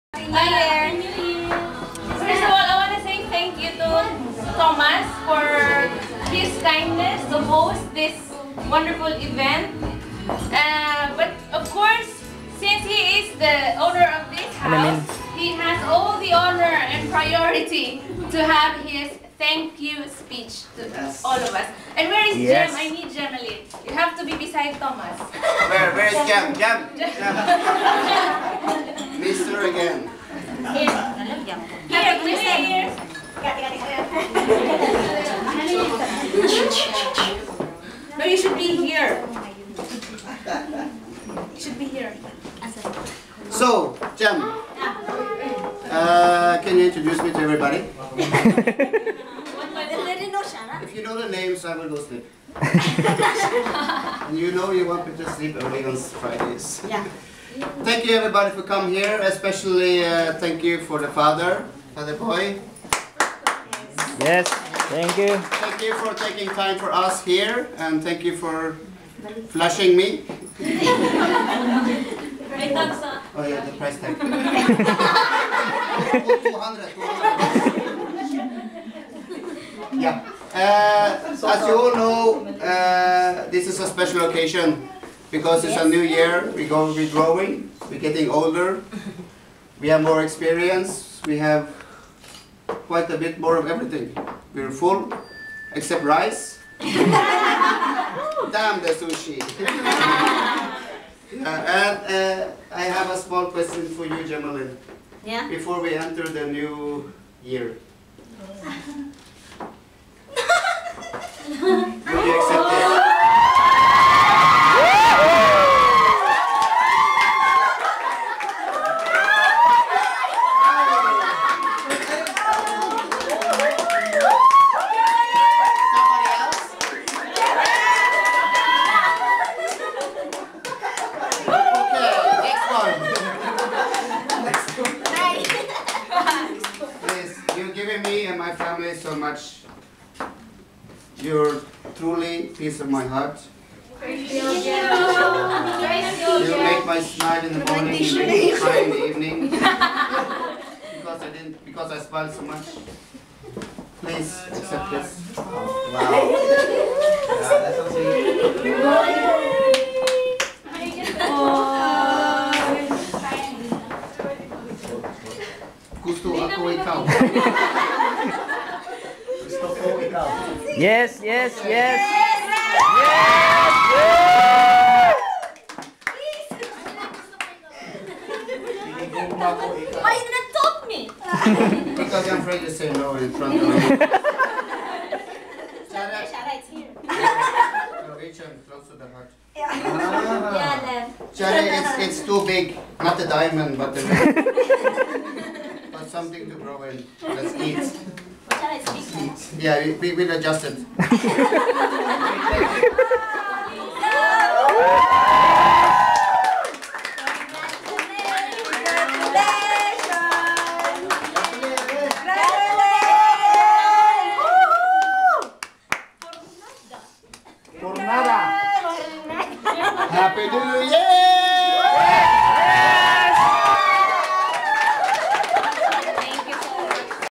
Proposal of thomas to jemalyn
December 31 2010 at 8:15 thomas proposed to our dearly friend jemalyn..oslo norway...